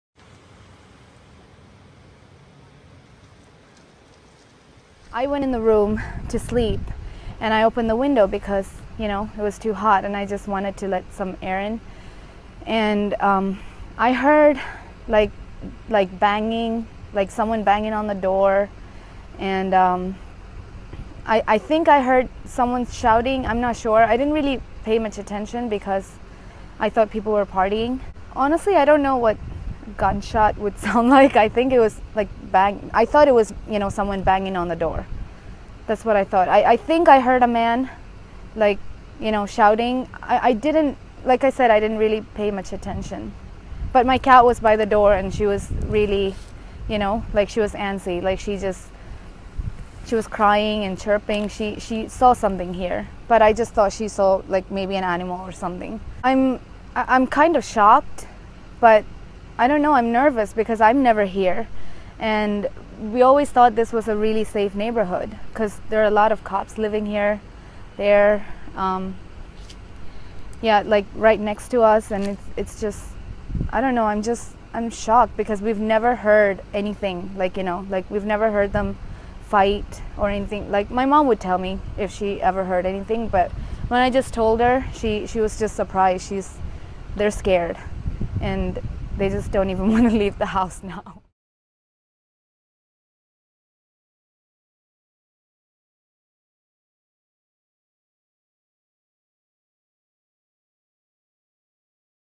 Video: Murder-suicide shocks Warner Robins neighbors
Warner Robins neighbors in Tyson Glen woke up to news of a murder-suicide Valentine's night. Police say 41-year-old Christopher Hankinson shot and killed his girlfriend, 45-year-old Angela Polk, before turning the gun on himself. Video by Liz Fabian